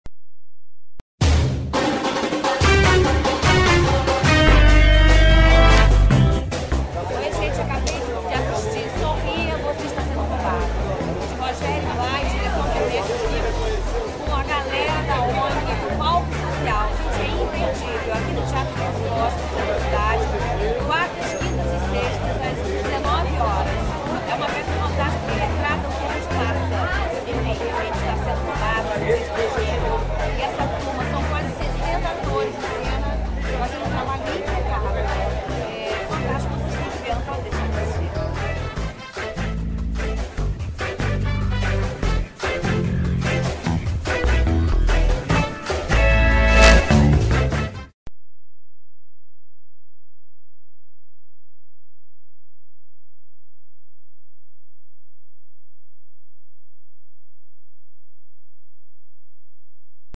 ZORRA TOTAL - Alexandra Richter - "Sorria, Você Está Sendo Roubado!"
A atriz e humorista do elenco de ZORRA TOTAL, Alexandra Richter, convidando o público à assistir a comédia musical "SORRIA, VOCÊ ESTÁ SENDO ROUBADO!". O espetáculo, de Rogério Blat e direção de Ernesto Piccolo, está arrebatando o público carioca de 4ª à 6ª feira sempre às 19h no Teatro Glauce Rocha. Contando a história de uma típica família brasileira, a peça fala sobre as cobranças abusivas, altas taxas e impostos além da insegurança pública com muito humor e descontração.

TEATRO GLAUCE ROCHA
(em frente o Metrô da Carioca e acessível pela Rua da Ajuda)